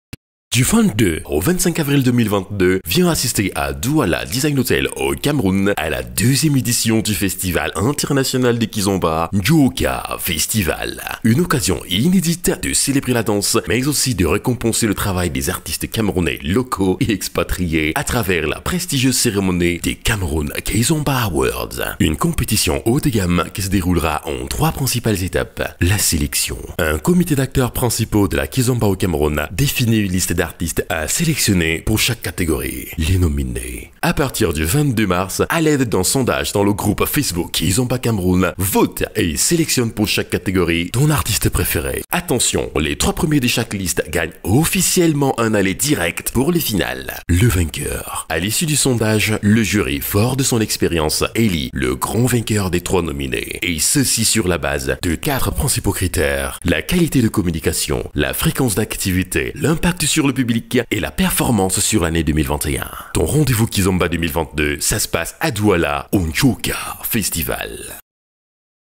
Djocka Festival ☆ Cameroon Kizomba Awards ░ Doula - Cameroun | 2022
Attention !
Du 23 au 25 Avril 2022 viens assister à Douala Design hôtel au Cameroun, à la deuxième édition du festival international de Kizomba, "Djocka Festival" une occasion inédite de célébrer la danse, mais aussi de récompenser le travail des artistes camerounais locaux et expatriés, à travers la prestigieuse cérémonie des "Cameroon Kizomba Awards"